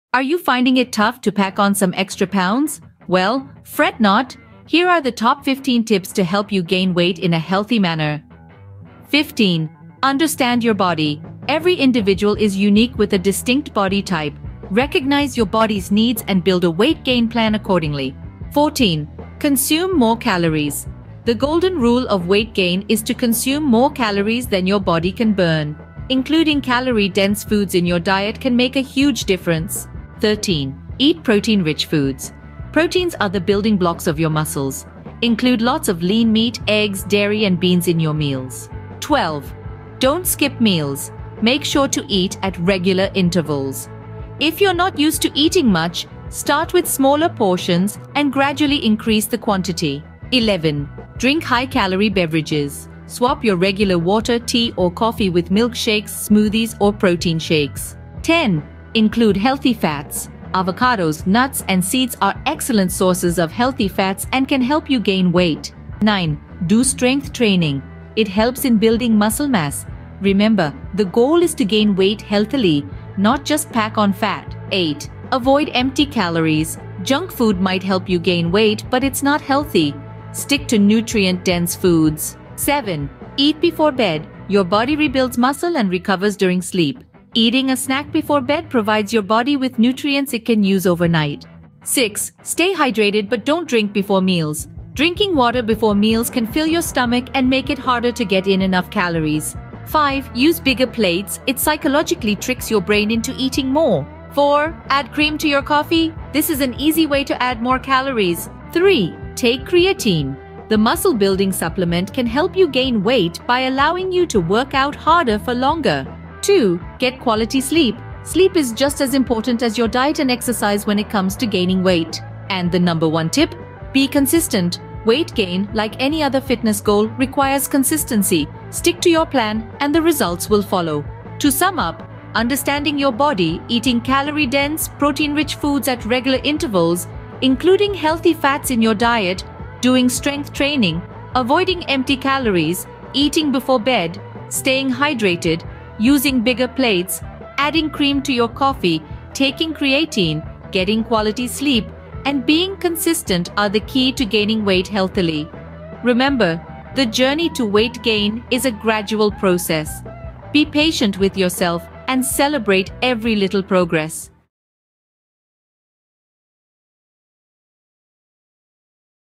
Top 15 Power Tips for Healthy Weight Gain
In this video, we're diving deep into the world of healthy weight gain. If you've been struggling to put on weight or simply want to do it in a sustainable way, you've come to the right place.

Join us as we present the top 15 tips for healthy weight gain, all backed by science and nutrition experts. From choosing the right foods to incorporating effective exercise routines, we've got you covered.

Discover the science behind weight gain and learn how to create a personalized meal plan that suits your body's needs. We'll share evidence-based strategies to increase muscle mass and boost your overall health.

Whether you're a fitness enthusiast, an athlete, or simply looking to achieve a healthier weight, these tips are guaranteed to help you on your journey. Get ready to make positive changes in your life and reach your weight gain goals.

Together, we can spread the knowledge and empower others to achieve their ideal weight.

OUTLINE:

00:00:00 Understanding Your Body
00:00:18 Consuming More Calories
00:00:30 Eating Protein-Rich Foods
00:00:38 Not Skipping Meals
00:00:50 Drinking High-Calorie Beverages
00:00:58 Including Healthy Fats
00:01:05 Doing Strength Training
00:01:14 Avoiding Empty Calories
00:01:22 Eating Before Bed
00:01:31 Staying Hydrated and Not Drinking Before Meals
00:01:39 Using Bigger Plates
00:01:44 Adding Cream to Your Coffee
00:01:48 Taking Creatine
00:01:56 Getting Quality Sleep
00:02:02 Being Consistent
00:02:11 Summary and Conclusion